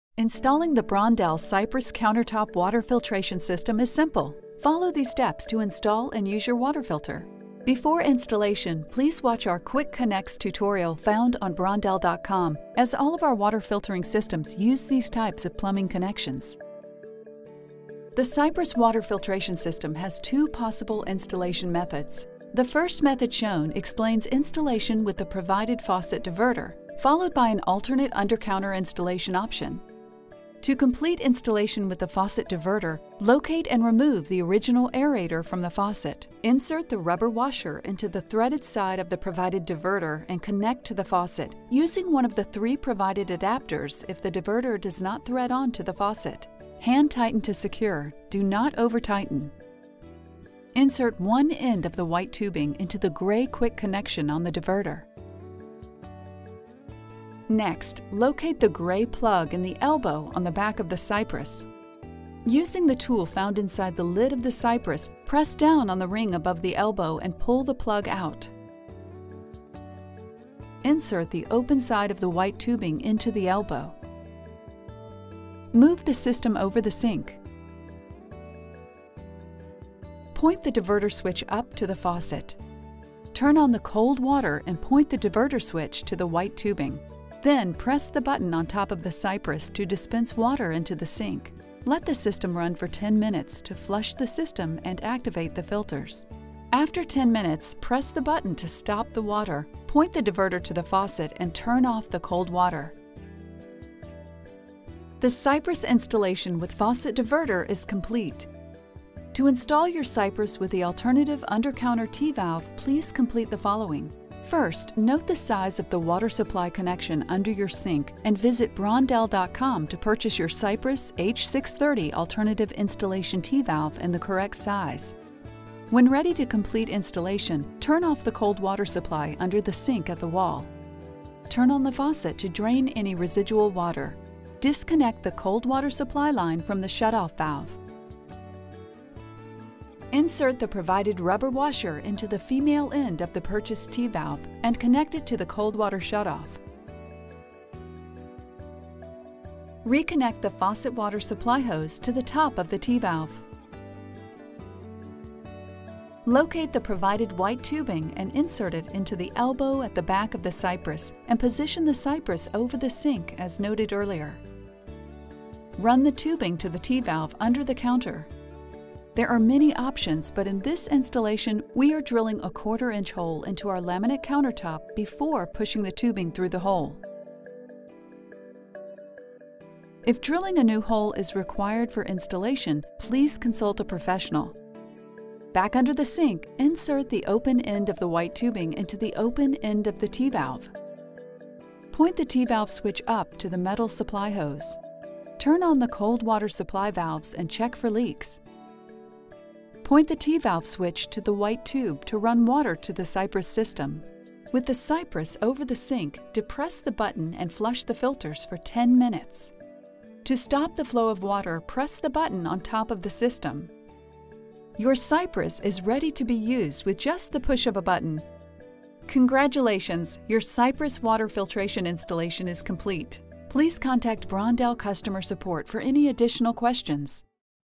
Brondell Cypress Triple-Stage Countertop Water Filter (H630) Installation Guide
The H2O+ Cypress installs in under 10 minutes with the provided faucet diverter and fits 99% of home faucets (note: not compatible with “integrated pull-out or pull-down sprayer” type faucets). Alternatively, you can install the water connection under your sink with an available T-valve (sold separately).

In about the same time and ease as changing a light bulb, you can have continuous refreshing filtered water at the push of a button — making this the fastest, easiest, and most elegant high-quality filtration system available.